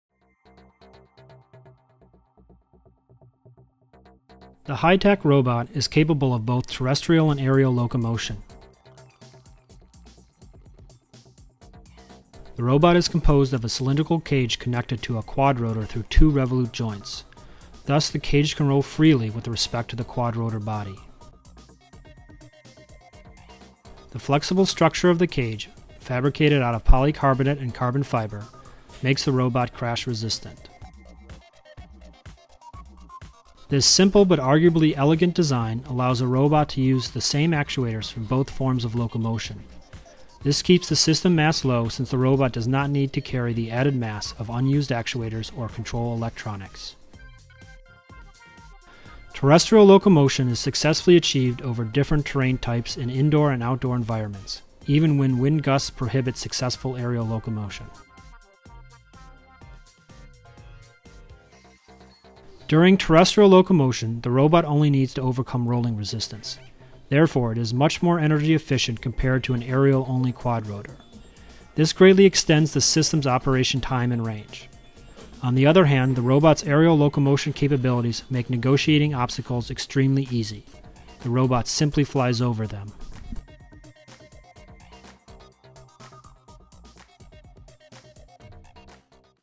HyTAQ Robot
HyTAQ Robot (Hybrid Terrestrial and Aerial Quadrotor)
Arash Kalantari, Matthew Spenko

The HyTAQ robot has been developed in the Robotics Lab at Illinois Institute of Technology (IIT), part of the Mechanical, Materials, and Aerospace Engineering Department. It is a novel mobile robot capable of both aerial and terrestrial locomotion. Flight is achieved through a quadrotor configuration; four actuators provide the required thrust. Adding a rolling cage to the quadrotor makes terrestrial locomotion possible using the same actuator set and control system. Thus, neither the mass nor the system complexity is increased by inclusion of separate actuators for terrestrial and aerial locomotion.

During terrestrial locomotion, the robot only needs to overcome rolling resistance and consumes much less energy compared to the aerial mode. This solves one of the most vexing problems of quadrotors and rotorcraft in general — their short operation time. Experimental results show that the hybrid robot can travel a distance 4 times greater and operate almost 6 times longer than a aerial only system. It also solves one of the most challenging problems in terrestrial robot design — obstacle avoidance. When an obstacle is encountered, the system simply flies over it.